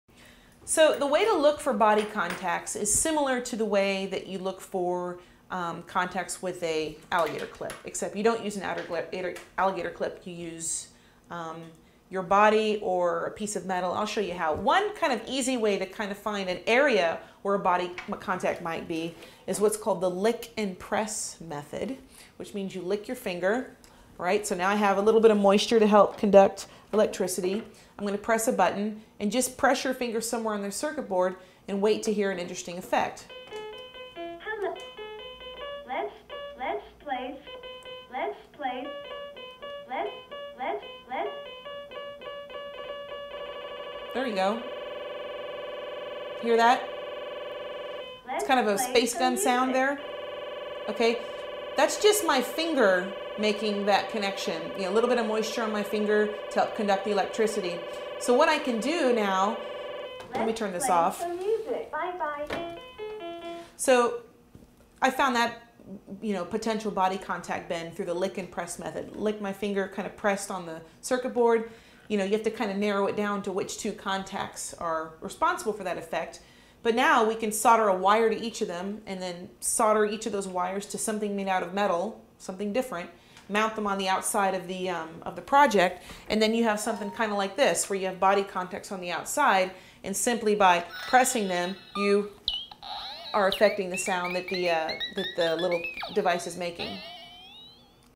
How to Do Circuit Bending with a Kid's Toy : The Lick & Press Finger Method for Circuit Bending
Learn the lick and press method with expert tips and advice on circuit bending in this free video clip on noise music.